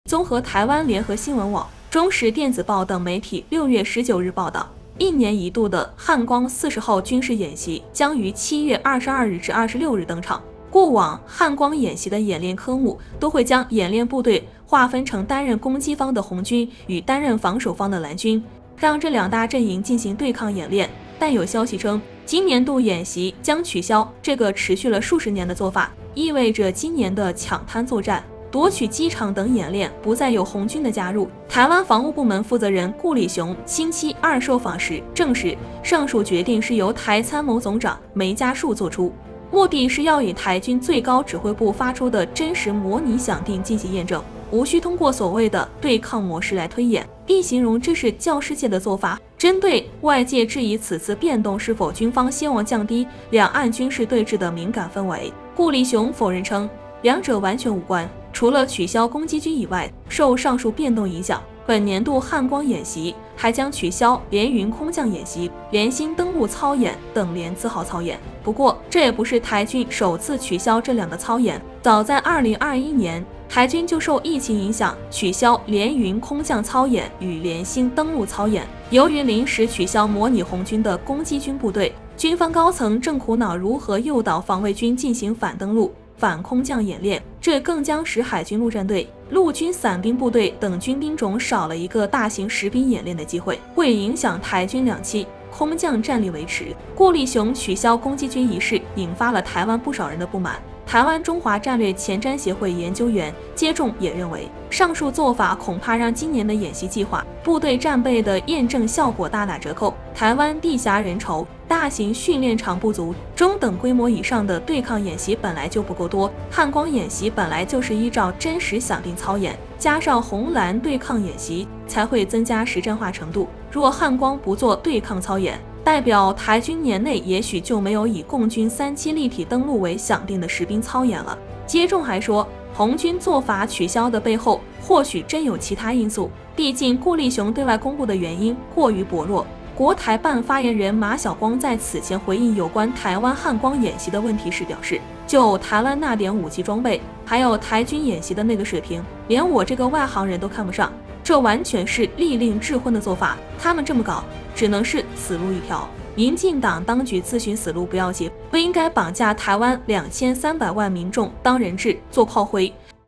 取消“攻击军”！台军“汉光40号”演习“小红帽”没了
，一年一度的“汉光40号”军事演习将于7月22日至26日登场。过往汉光演习的演练科目都会将演练部队划分成担任攻击方的“红军”与担任防守方的“蓝军”，让这两大阵营进行对抗演练。